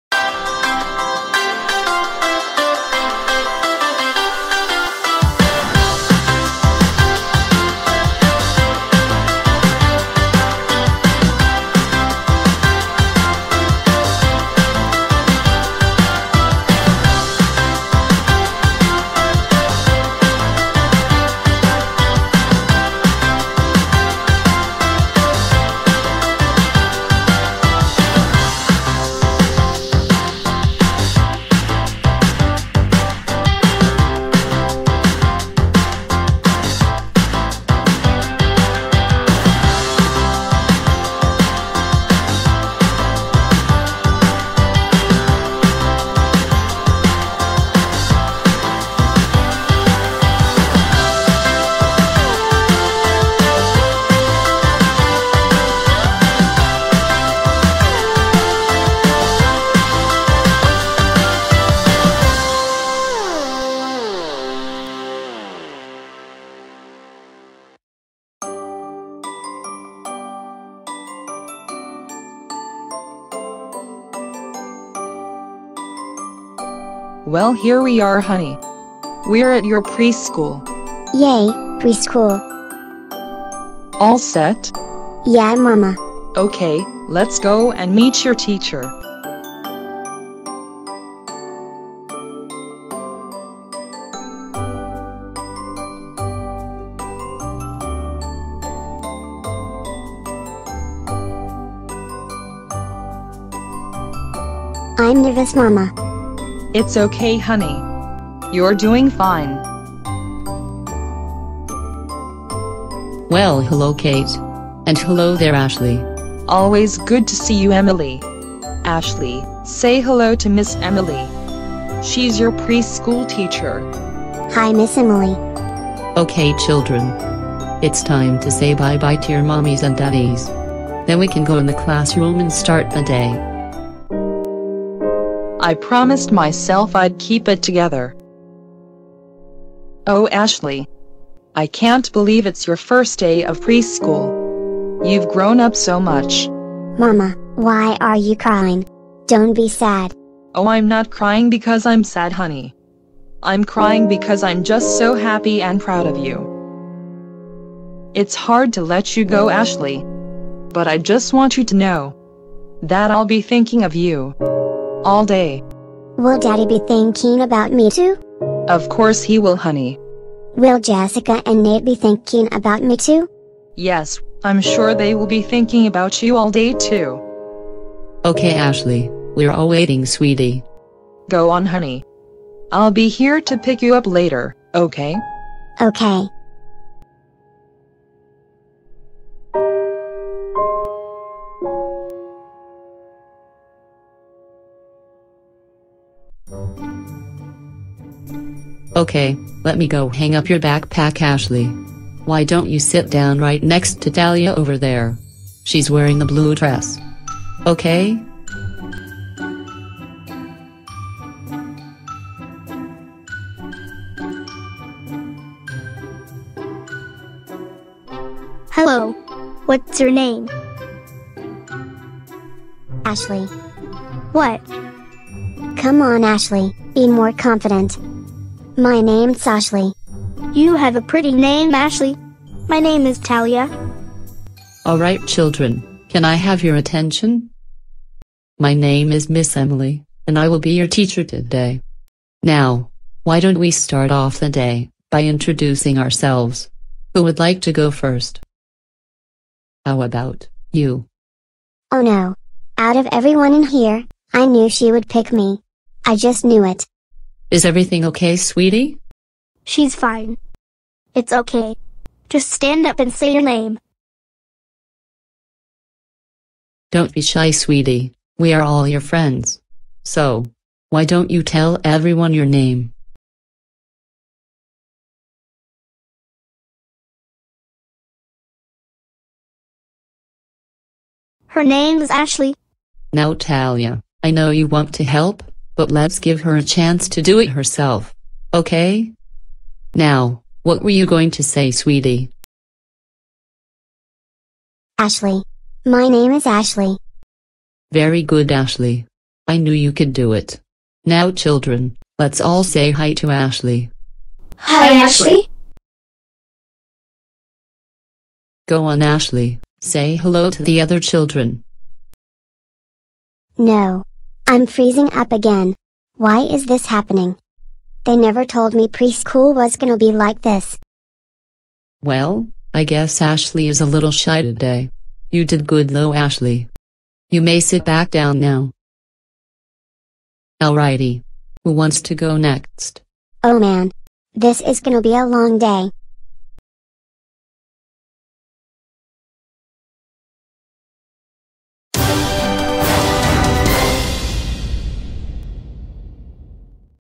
The Jessica Andrews Show (Episode 68: Preschool Daze)
We saw Ivy Collins' first day of kindergarten on "The Collins Family." Now, we see Ashley's first day of preschool. But her first day doesn't seem to go as smoothly.
Story treatment  by nicktoper.